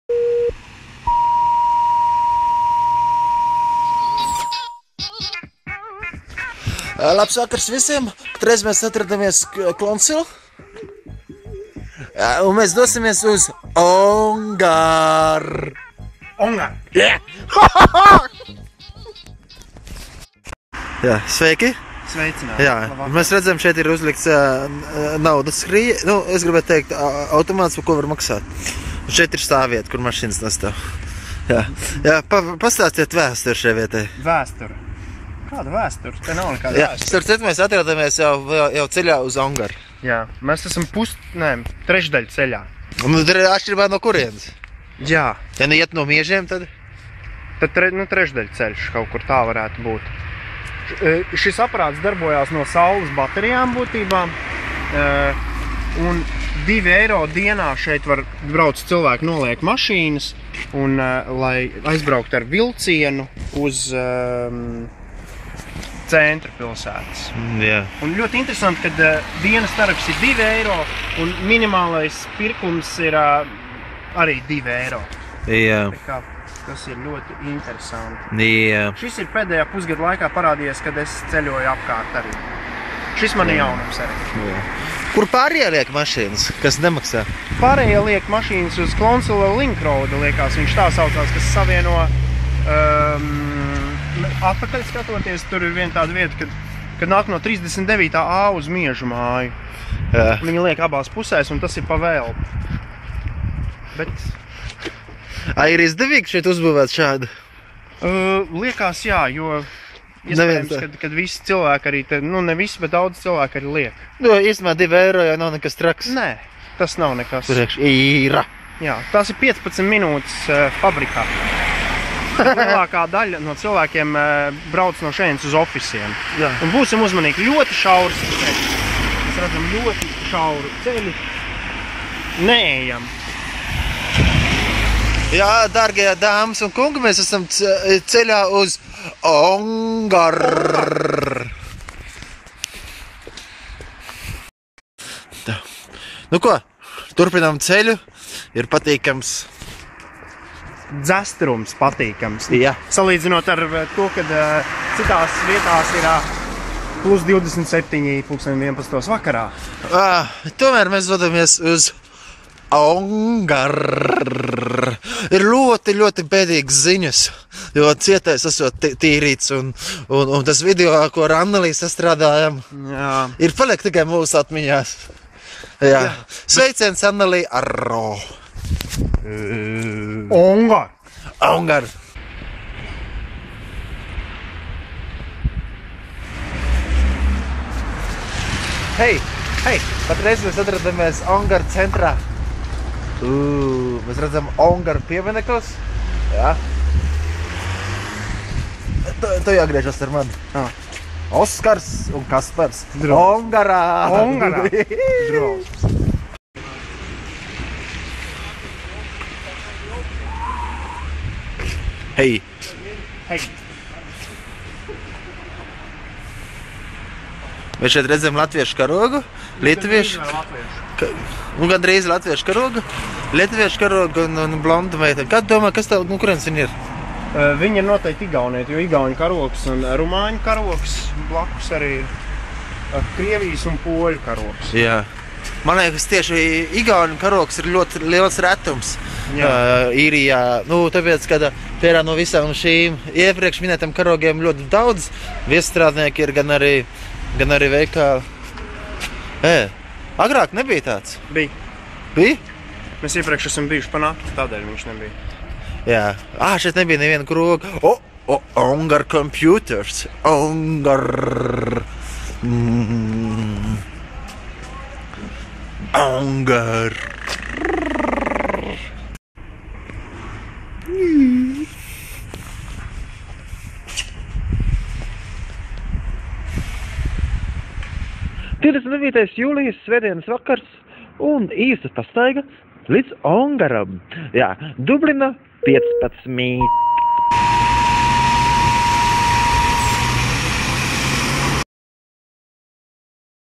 ONGAR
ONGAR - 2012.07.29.

Kaspars Ezerins ( aka Kamielitis )
& Oskars Svilans ekspedicijaa uz ONGAR

no kamielisha emurára!